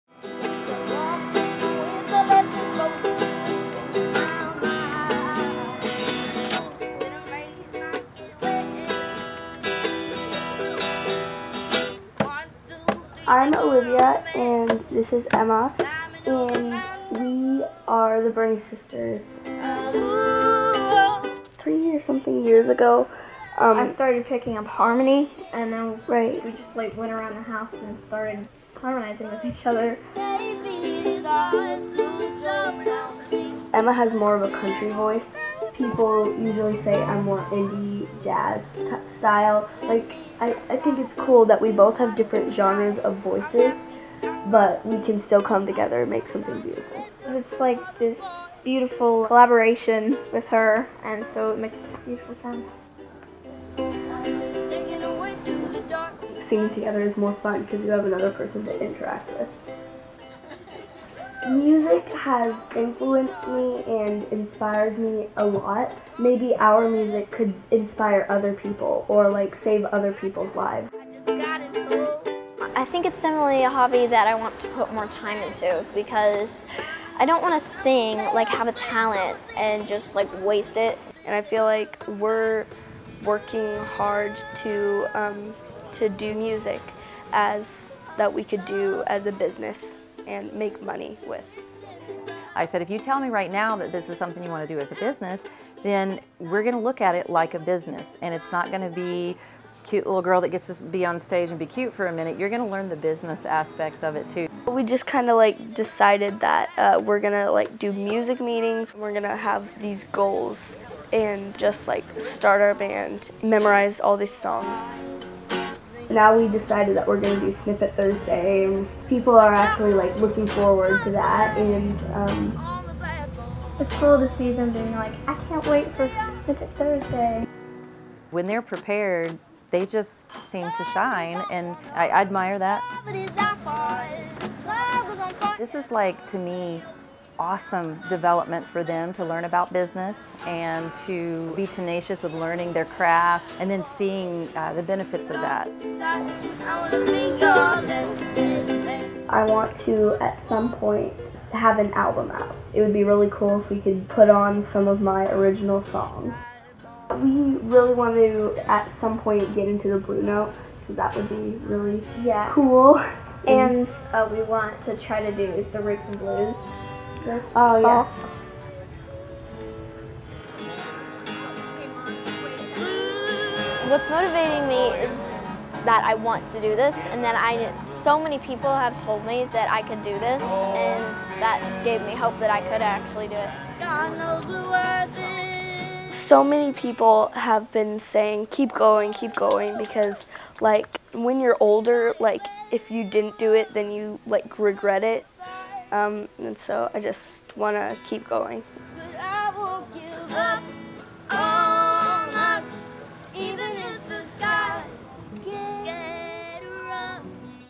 Meet Columbia's youngest musical stars: The Burney Sisters
Emma and Olivia Burney, aged 9 and 11 respectively, perform music in Columbia as the Burney Sisters. Their remarkable voices have been inspiring locals for some time now, and the sisters show no signs of stopping. In this video, we take an in-depth look at how the Burney Sisters manage their music and their business. Produced by Songxin Xie.